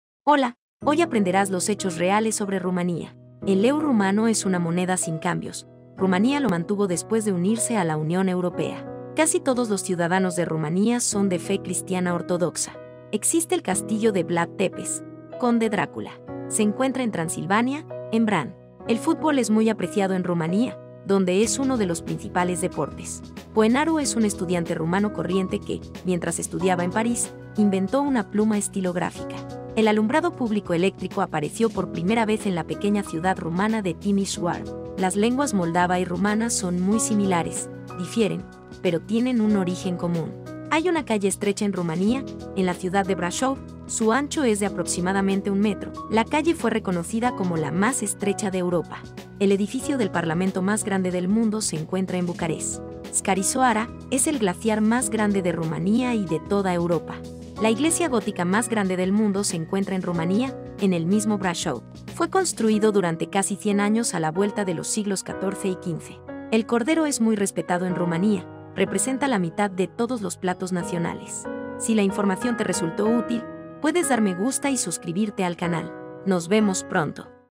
¡HECHOS REALES SOBRE RUMANÍA!
Hola,  hoy aprenderás los HECHOS REALES SOBRE RUMANÍA!  El leu rumano es una moneda sin cambios,  Rumania lo mantuvo después de unirse a la Unión Europea.  Casi todos los ciudadanos de Rumanía son de fe cristiana ortodoxa.  Existe el castillo de Vlad Tepes (Conde Drácula).  Se encuentra en Transilvania,  en Bran.  El fútbol es muy apreciado en Rumanía,  donde es uno de los principales deportes.  Poenaru es un estudiante rumano corriente que,  mientras estudiaba en París,  inventó una pluma estilográfica.  El alumbrado público eléctrico apareció por primera vez en la pequeña ciudad rumana de Timisoara.  Las lenguas moldava y rumana son muy similares,  difieren,  pero tienen un origen común.  Hay una calle estrecha en Rumania,  en la ciudad de Brasov,  su ancho es de aproximadamente un metro,  la calle fue reconocida como la más estrecha de Europa.  El edificio del parlamento más grande del mundo se encuentra en Bucarest.  "Scarisoara" es el glaciar más grande de Rumanía y de toda Europa.  La iglesia gótica más grande del mundo se encuentra en Rumania,  en el mismo Brasov.  Fue construido durante casi cien años a la vuelta de los siglos XIV y XV.  El cordero es muy respetado en Rumanía,  representa la mitad de todos los platos nacionales.  Nos vemos pronto.